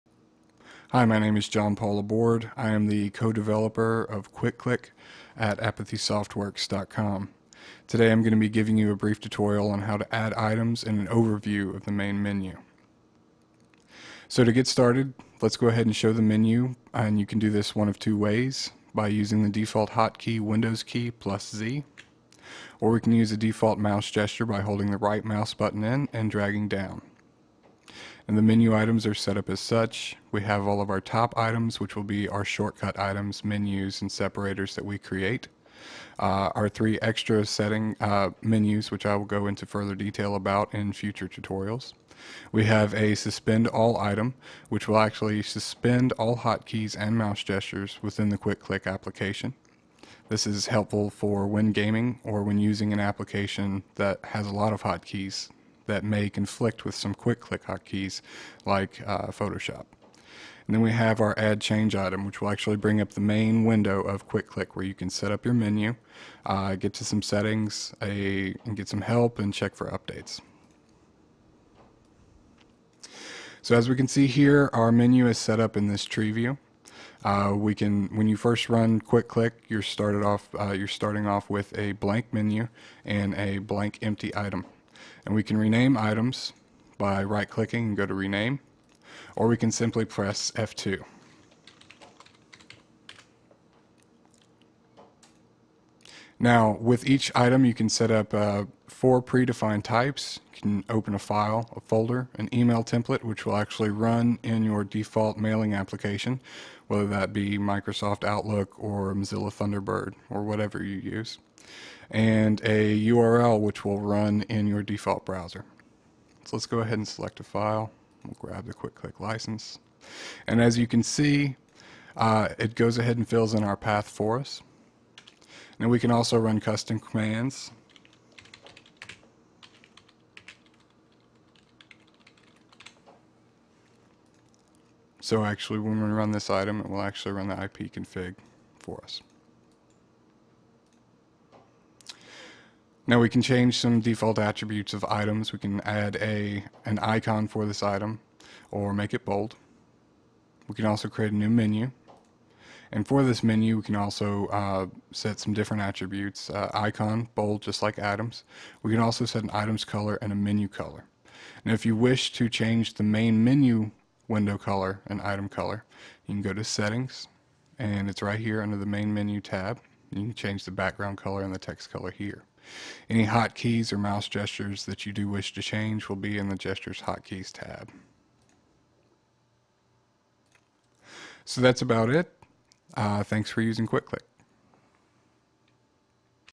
NANY 2011: Quick Cliq
This video is an overview of the NANY 2011 entry Quick Cliq.

Quick Cliq is a Windows program that speeds up your daily computer activities by giving you links to folders, files, programs, URLs and command line operations. It also offers window management, extreme clipboard manipulation, and note taking capabilities. All of this is provided in a compact menu that you can show anywhere and within any application or window through customizable hot keys and mouse gestures.